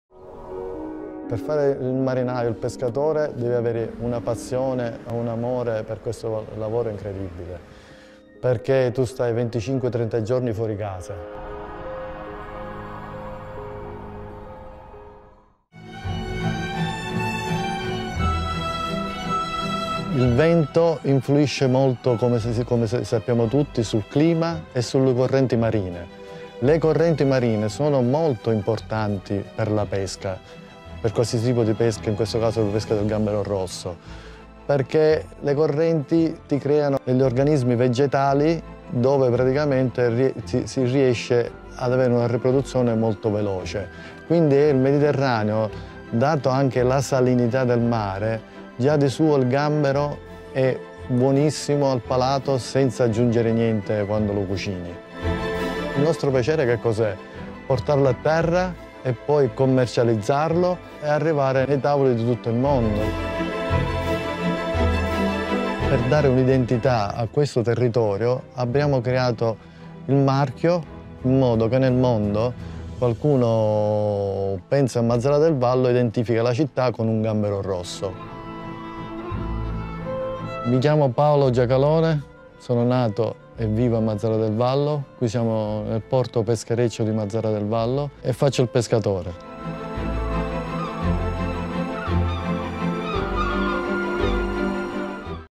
Per fare il pescatore devi avere un’incredibile amore e passione...”
Rosso di Mazara® prende parte al progetto di sostenibilità all'insegna della HumanEnergy condiviso da edisonenergia e vanityfair , che racconta la connessione tra i vari elementi della natura, ciò che rende il nostro pianeta vivo.